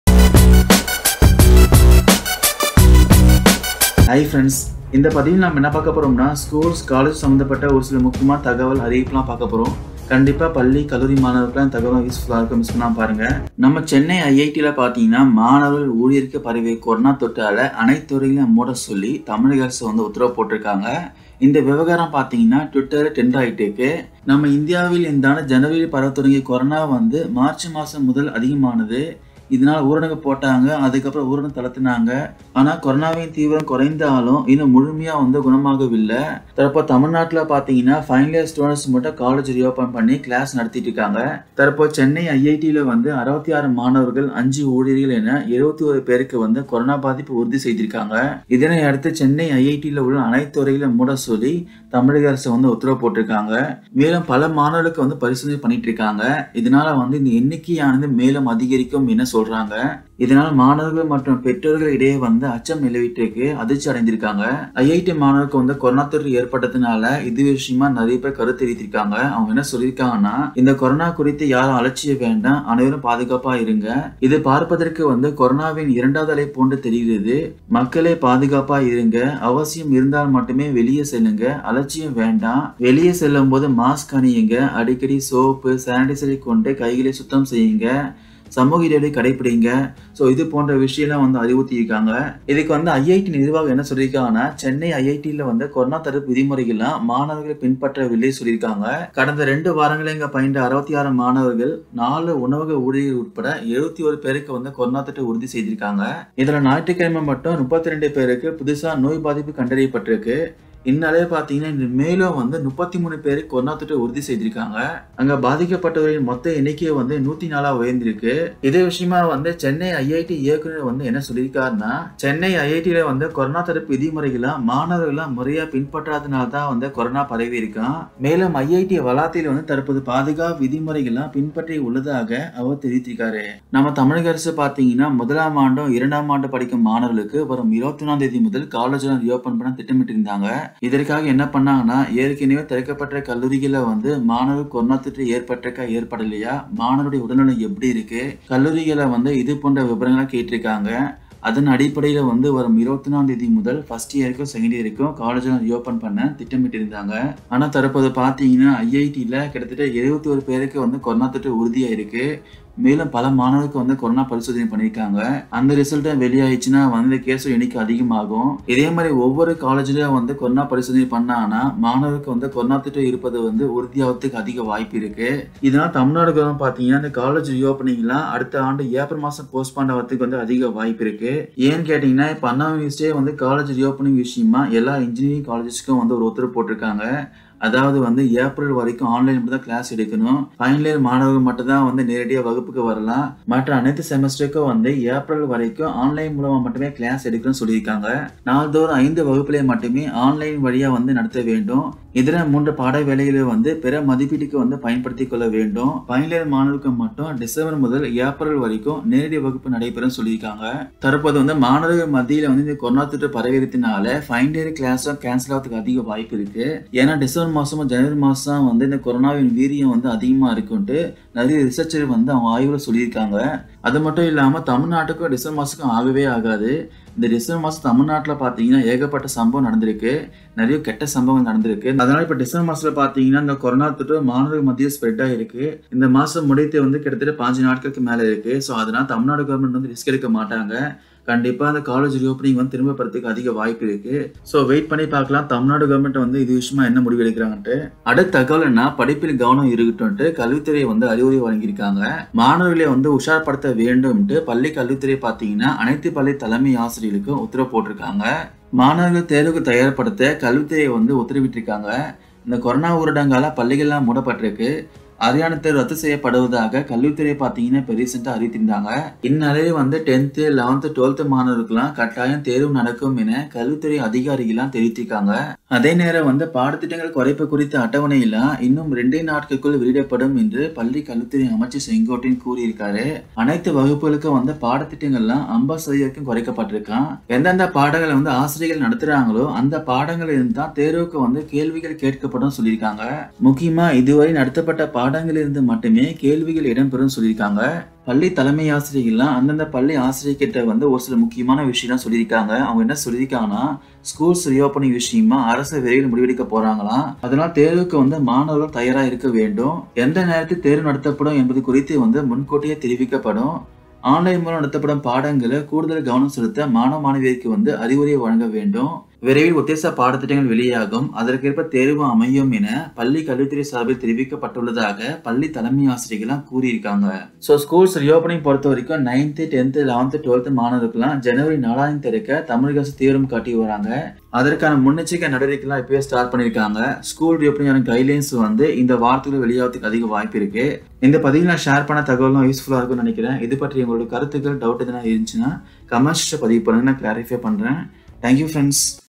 மாணவர்கள் மத்தியில் பரவிய கொரோனா அனைத்து கல்லூரி மூட உத்தரவு🔥 ஏப்ரல் வரை பள்ளி கல்லூரி திறப்பு இல்லை
🔥மாணவர்கள் மத்தியில் பரவிய கொரோனா அனைத்து கல்லூரிகள் மூட உத்தரவு! 🔥ஏப்ரல் 2021 வரை கல்லூரிகள் திறப்பு இல்லை! 🔥பள்ளி மாணவர்களுக்கு கல்வித்துறை பிறப்பித்த புதிய உத்தரவு! 🔥பள்ளிகள் திறப்பு உறுதி செய்த கல்வித்துறை

🔥 படிப்பில் கவனம் இருக்கட்டும் கல்வித்துறை அறிவுரை: மாணவர்களை உஷார்படுத்தும் கல்வித்துறை

மாணவர்களை தேர்வுக்கு தயார்படுத்த, கல்வித்துறை உத்தரவிட்டுள்ளது.கொரோனா ஊரடங்கால் பள்ளிகள் மூடப்பட்டுள்ள நிலையில், அரையாண்டு தேர்வு ரத்து செய்யப்படுவதாக, கல்வித்துறை சார்பில் சமீபத்தில் அறிவிக்கப்பட்டது.

இந்நிலையில், 10, 11 மற்றும் பிளஸ் 2 மாணவர்களுக்கு, கட்டாயம் தேர்வு நடக்கும் என, தெரிவிக்கப்பட்டுள்ளது.அதே நேரம், பாடத்திட்டம் குறைக்கப் படுவதும், உறுதி செய்யப்பட்டுள்ளது. இதுவரை நடத்தப்பட்ட பாடங்களில் இருந்து மட்டுமே கேள்விகள் இடம் பெறும் என தெரிவிக்கப்பட்டுள்ளதால், மாணவர்கள் ஆறுதல் அடைந்துள்ளனர்.

தலைமையாசிரியர்கள் சிலர் கூறியதாவது;

பள்ளி திறப்பு குறித்து, அரசு விரைவில் முடிவெடுக்க உள்ளது. தேர்வுக்கு மாணவர்கள் தயாராக இருக்க வேண்டும்.

எந்நேரத்தில் தேர்வு நடத்தப்படும் என்பது குறித்து முன்கூட்டியே தெரிவிக்கப்படும். ஆன்லைன் மூலம் நடத்தப்படும் பாடங்களில், கூடுதல் கவனம் செலுத்த மாணவ, மாணவியருக்கு அறிவுரை வழங்க வேண்டும்.

விரைவில் உத்தேச பாடத்திட்டம் வெளியாகும்; அதற்கேற்ப தேர்வும் அமையும் என, பள்ளி கல்வித்துறை சார்பில் தெரிவிக்கப்பட்டுள்ளது.இவ்வாறு, அவர்கள் கூறினர்

🔥சென்னை ஐஐடி மாணவர்களுக்கு கொரோனா : டுவிட்டரில் டிரெண்டிங்

சென்னை ஐஐடி.,யில் மாணவர்கள், ஊழியர்களுக்கு பரவிய கொரோனா தொற்றால் அனைத்து துறைகளையும் மூடப்பட்டுள்ளது. இந்த விவகாரம் டுவிட்டரில் டிரெண்ட் ஆகிறது.

இந்தியாவில் இந்தாண்டு ஜனவரியில் பரவ தொடங்கிய கொரோனா, மார்ச் மாதம் முதல் அதிகமானது. இதனால் ஊரடங்கு பிறபிக்கப்பட்டது. பின்னர் ஊரடங்கு தளர்த்தப்பட்டது. கொரோனாவின் தீவிரம் குறைந்தாலும் இன்னும் முழுமையாக குணமாகவில்லை. தற்போது தமிழகத்தில் மூன்றாம் ஆண்டு மாணவர்களுக்கு கல்லூரிகள் திறக்கப்பட்டு செயல்பட்டு வருகின்றனர்.

இந்நிலையில் சென்னை ஐஐடி.,யில் 66 மாணவர்கள், 5 ஊழியர்கள் என 71 பேருக்கு கொரோனா பாதிப்பு உறுதி செய்யப்பட்டுள்ளது. இதனையடுத்து சென்னை ஐ.ஐ.டி.யில் உள்ள அனைத்து துறைகளையும் மூட உத்தரவு பிறப்பிக்கப்பட்டு உள்ளது. மேலும் பல மாணவர்களுக்கு பரிசோதனை நடக்கிறது. இதனால் இந்த எண்ணிக்கை மேலும் அதிகமாகும் என தெரிகிறது. இதனால் மாணவர்கள் மற்றும் பெற்றோர்கள் இடையே அச்சம் நிலவுகிறது.

ஐஐடி., மாணவர்களுக்கு கொரோனா என்ற செய்தி டுவிட்டரில் IITMadras என்ற ஹேஷ்டாக் உடன் டிரெண்ட் ஆனது. டுவிட்டரில் பலரும், பாதிக்கப்பட்டவர்கள் அனைவரும் சீக்கிரம் குணமடைய பிரார்த்திப்பதாக கருத்து பதிவிட்டுள்ளனர். இன்னும் சிலர், ''கொரோனா குறித்த அலட்சியம் வேண்டாம், அனைவரும் பாதுகாப்பாக இருங்கள்'' என அறிவுரை கூறி வருகின்றனர். இன்னும் சிலரோ, ''இது பார்ப்பதற்கு கொரோனாவின் இரண்டாவது அலை போன்று தெரிகிறது. மக்களே பாதுகாப்பாக இருங்கள், அவசியம் இருந்தால் மட்டும் வெளியே செல்லுங்கள். அலட்சியம் வேண்டாம். வெளியே செல்லும்போது மாஸ்க் அணியுங்கள், அடிக்கடி சோப் அல்லது சானிடைசர் கொண்டு கைகளை சுத்தம் செய்யுங்கள். சமூக இடைவெளியை கடைபிடியுங்கள்'' என அறிவுறுத்தி உள்ளனர்.

🔥 சென்னை ஐஐடி.,யில் 104 பேருக்கு கொரோனா

சென்னை ஐஐடி.,யில் ஏற்கனவே 71 பேருக்கு கொரோனா தொற்று உறுதி செய்யப்பட்டுள்ள நிலையில் மேலும் 33 பேருக்கு புதிதாக தொற்று உறுதி செய்யப்பட்டுள்ளது.

சென்னை ஐஐடி.,யில் 66 மாணவர்கள், 5 ஊழியர்கள் என 71 பேருக்கு கொரோனா பாதிப்பு உறுதி செய்யப்பட்டுள்ளது. இதனையடுத்து சென்னை ஐ.ஐ.டி.யில் உள்ள அனைத்து துறைகளையும் மூட உத்தரவு பிறப்பிக்கப்பட்டு உள்ளது. ஆராய்ச்சி மாணவர்கள் ஆன்லைன் வழியில் படிக்க அறிவுறுத்தப்பட்டு உள்ளது. மறு உத்தரவு வரும் வரை எந்த துறைகளும் செயல்படக் கூடாது எனவும் உத்தரவிடப்பட்டுள்ளது.